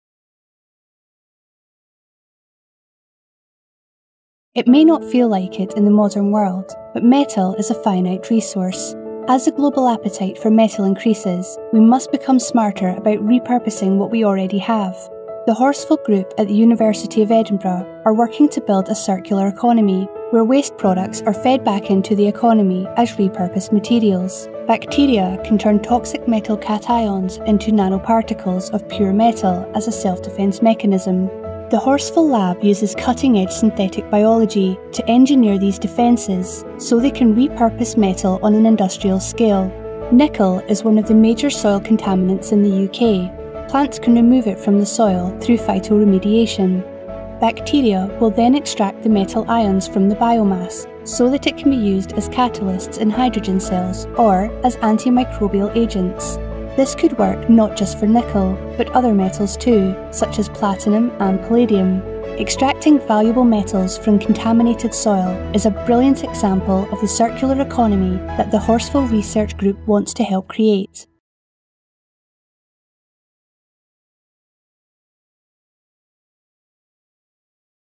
BiUP: Biological upcycling of metals
Exciting synthetic biology research which is helping the Horsfall Group at the University of Edinburgh contribute to the circular economy.